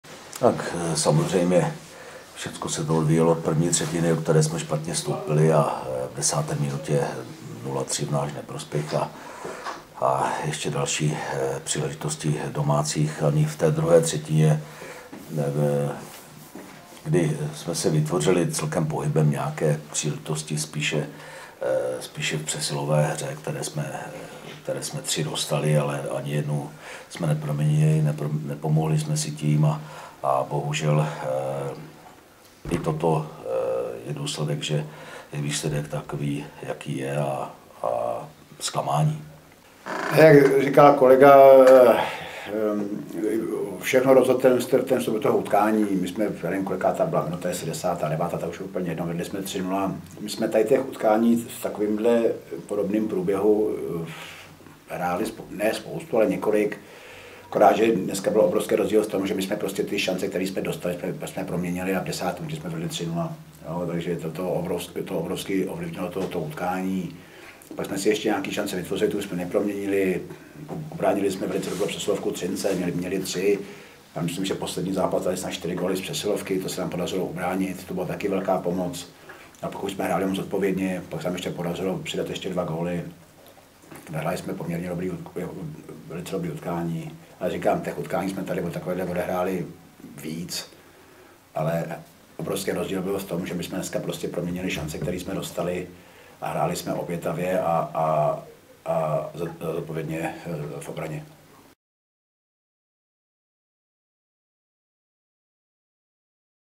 Trenéři po zápase: Rytíři Kladno - HC Oceláři Třinec: Pavel Skrbek a Zdeněk Moták
Tisková konference po utkání 49. kola Tipsport extraligy, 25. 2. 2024.